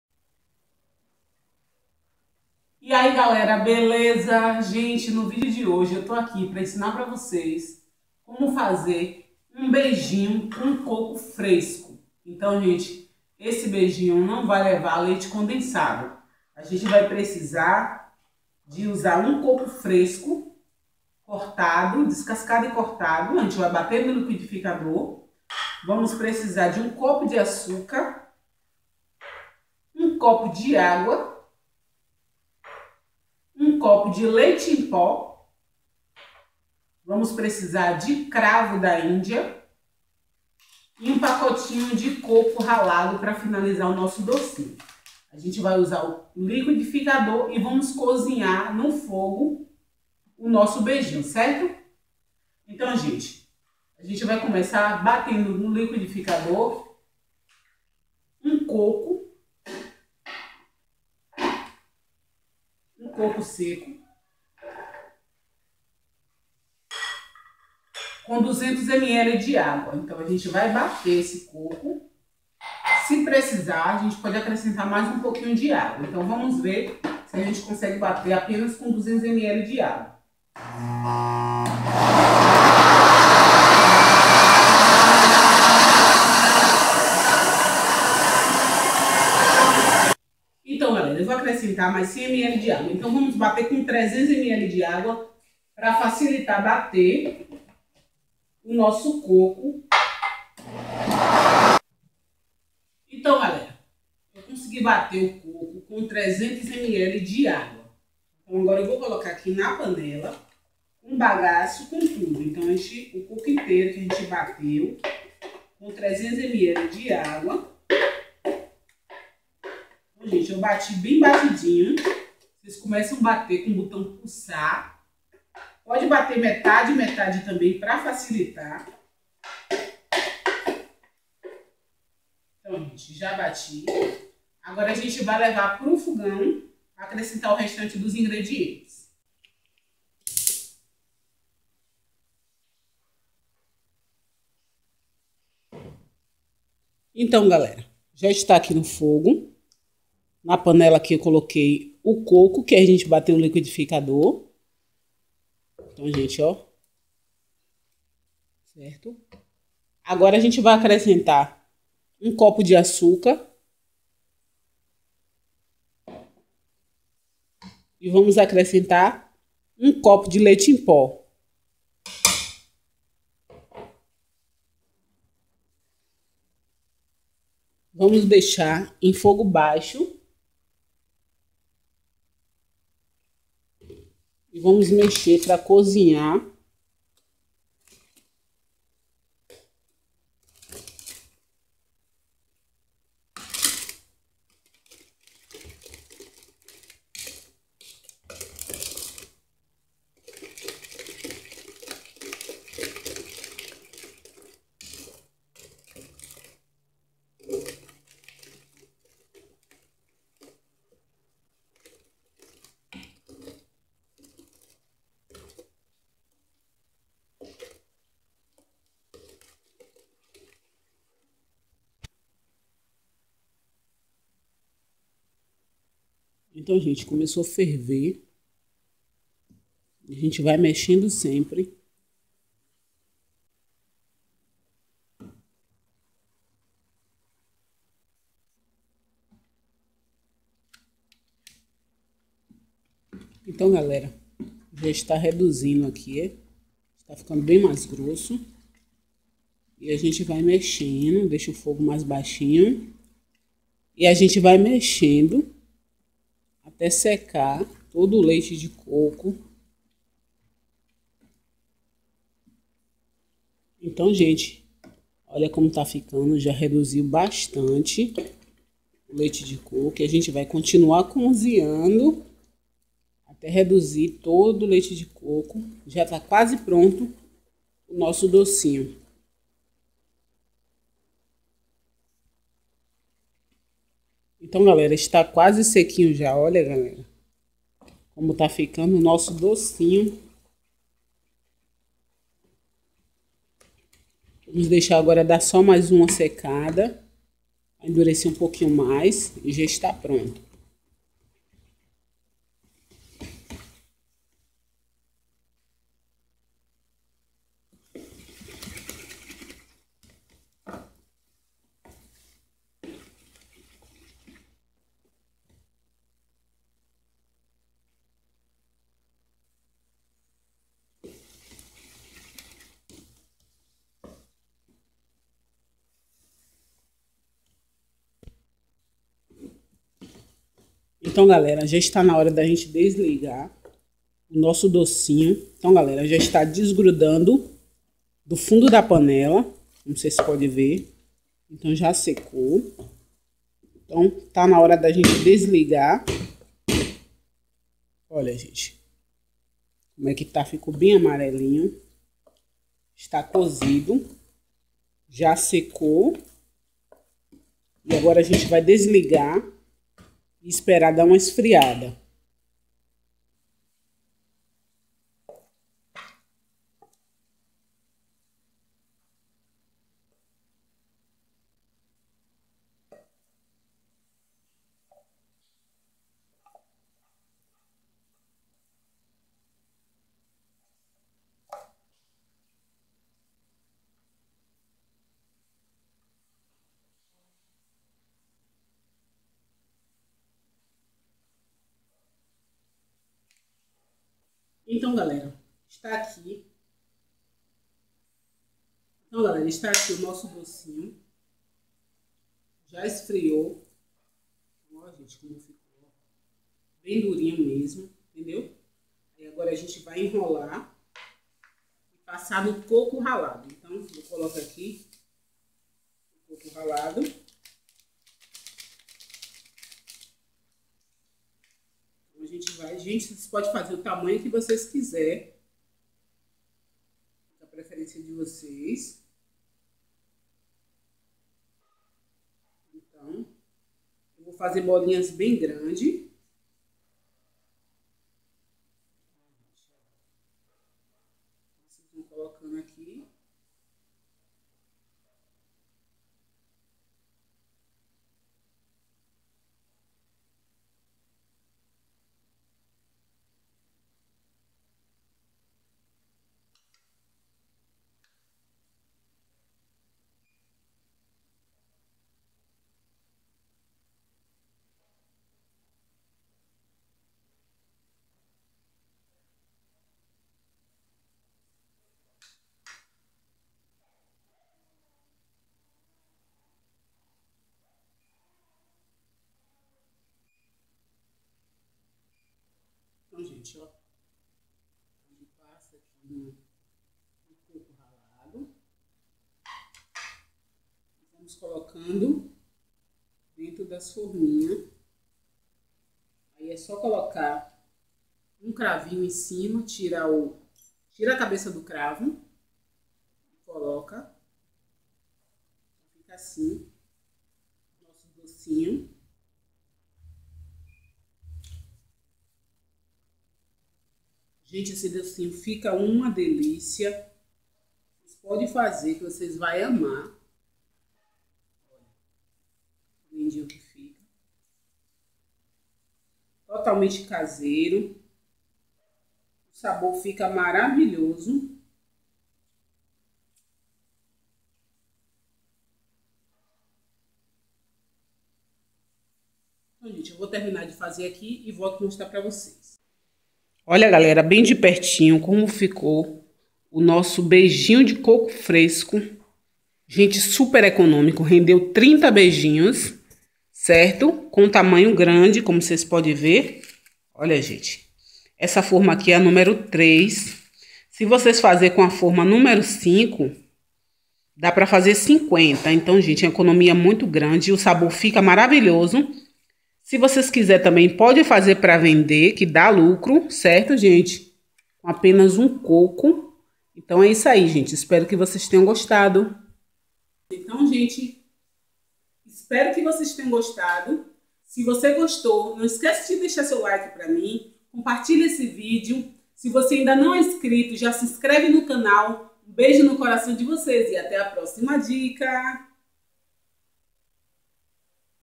BEIJINHO DE COCO SEM LEITE CONDENSADO
OLA GALERA MAIS UMA RECEITINHA PRA VOCES ESPERO QUE GOSTEM

RECEITA

1 COPO DE LEITE EM PÓ
1COPO DE AÇUCAR
300 ML DE AGUA
1 COCO SECO GRANDE
1 PACT DE ₢OCO RALADO
CRAVO DA INDIA